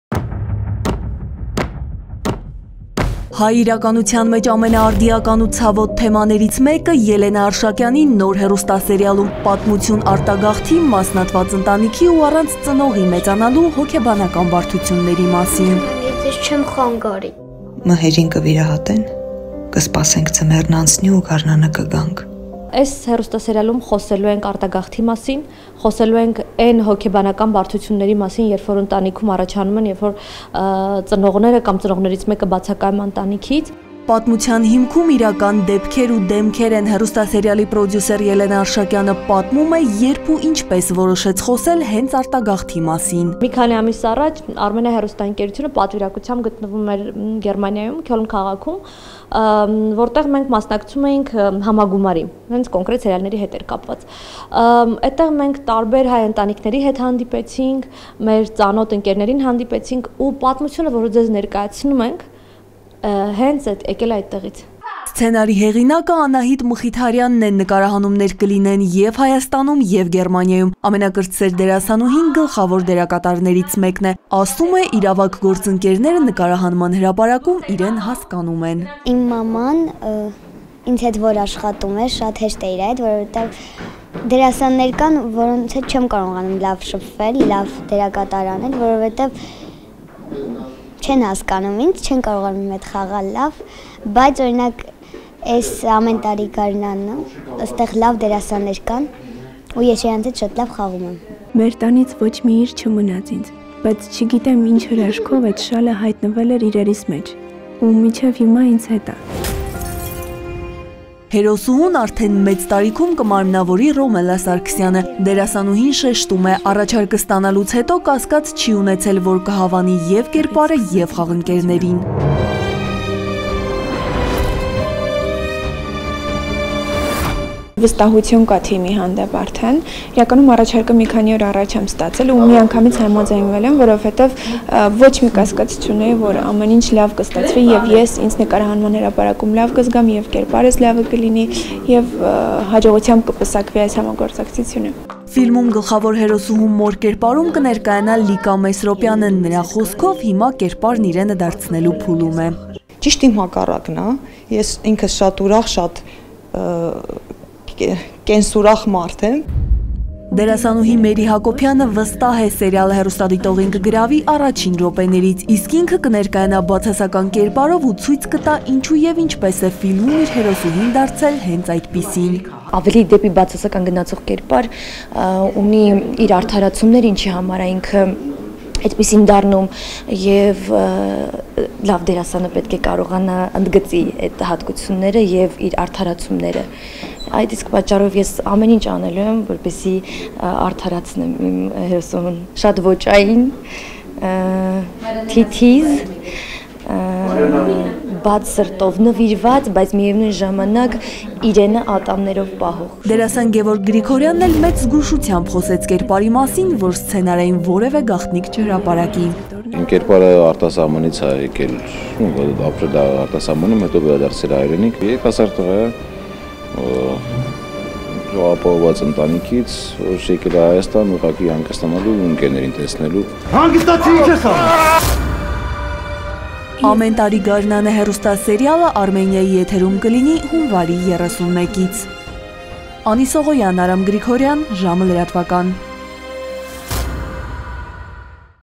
«Ամեն տարի՝ գարնանը»․ Արտագաղթի թեմաներով հեռուստասերիալը Արմենիա TV-ի եթերում կլինի հունվարի 31-ից
Արմենիա TV-ի եթերում կլինի նոր հեռուստասերիալ` «Ամեն տարի՝ գարնանը: Հեռուստապատմությունը ներկայացնում է արտագաղթի ծանրությունը, մասնատված ընտանիքի դժվարությունները: Սերիալի նկարահանումները տեղի կունենան Հայաստանում և Գերմանիայում: